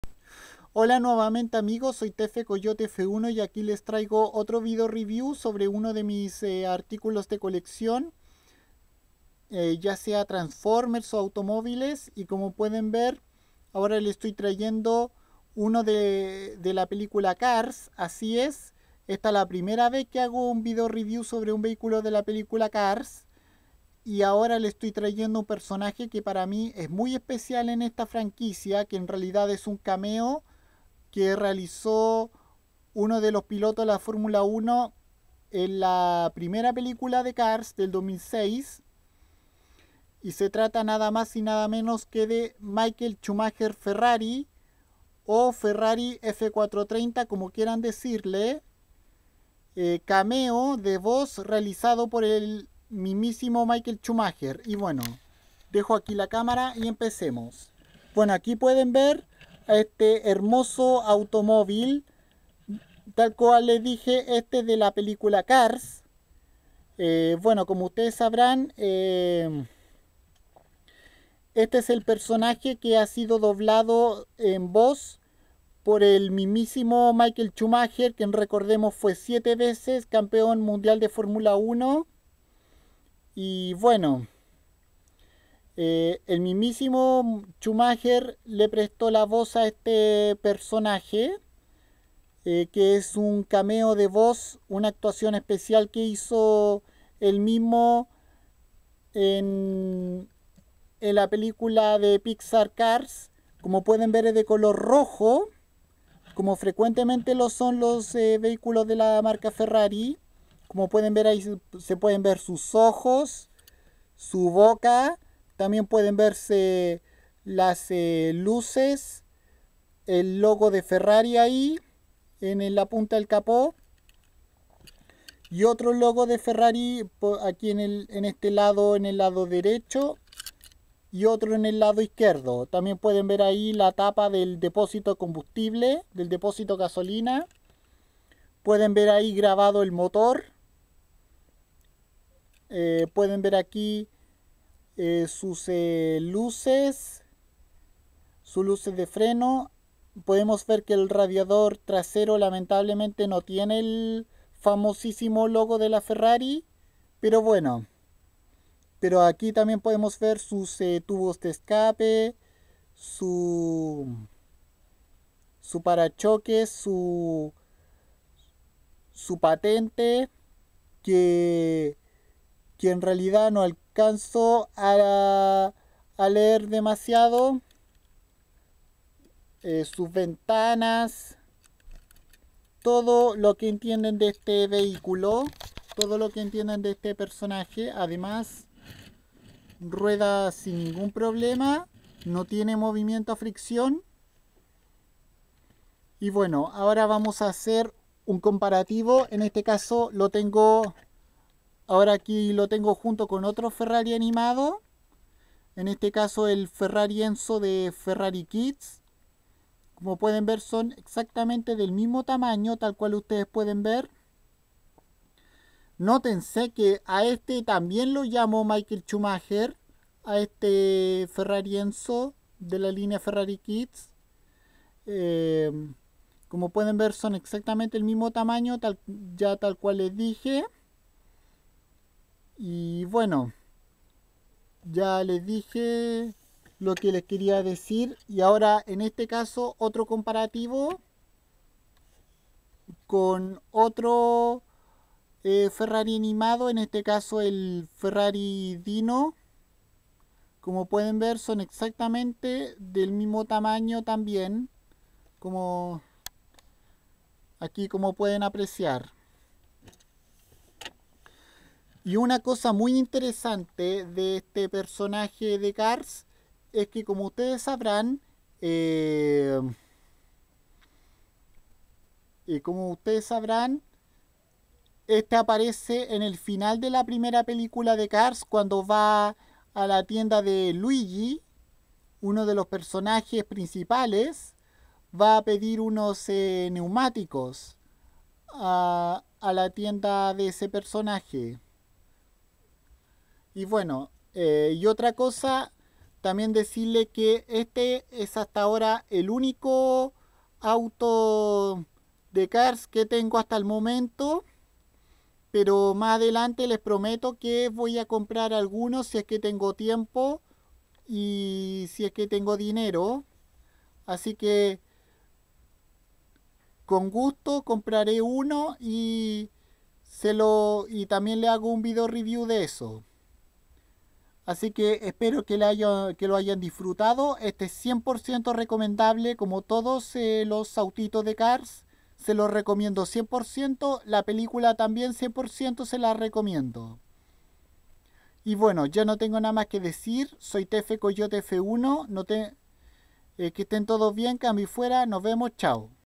Video Review en español Cars Pixar Ferrari F430(Michael Schumacher Ferrari)
Este es mi cameo favorito de la película Cars y es el Ferrari F430 que aparece al final de primera película de la franquicia y que fue doblado en voz por el alemán 7 veces ganador de la Fórmula 1 Michael Schumacher...Por favor recuerden sus comentarios,me gusta y suscribirse también...No se permiten comentarios ofensivos o malos porque los borro inmediatamente...Ademas es mi primer video-review sobre un autito de Cars